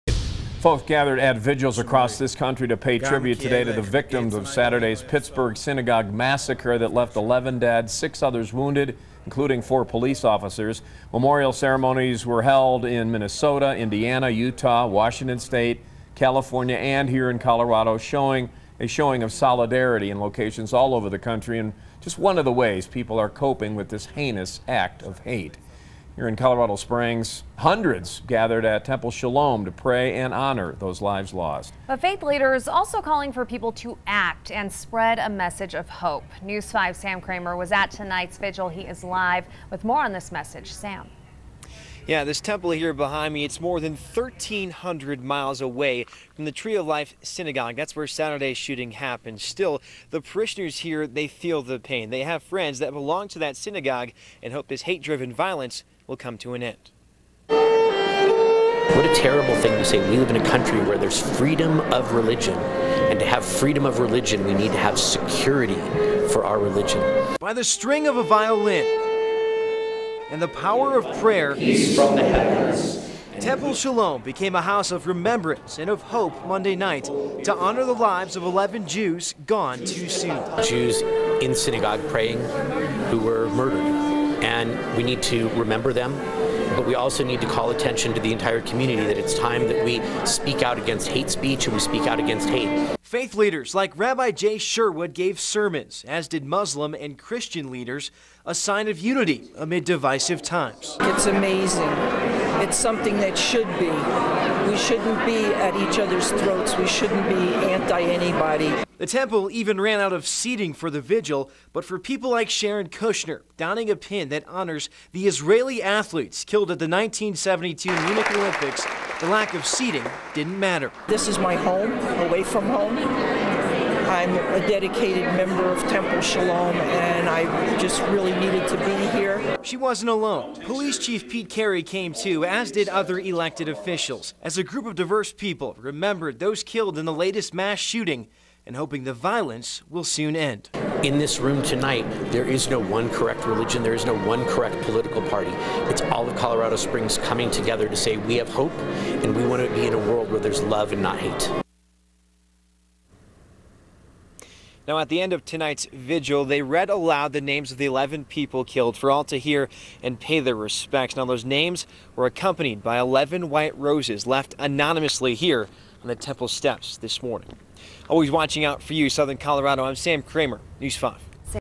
Hundreds gather to support Jewish community, honor shooting victims
"It's time to end hate speech and to show that we are better than hate. And love will always trump hate," he said.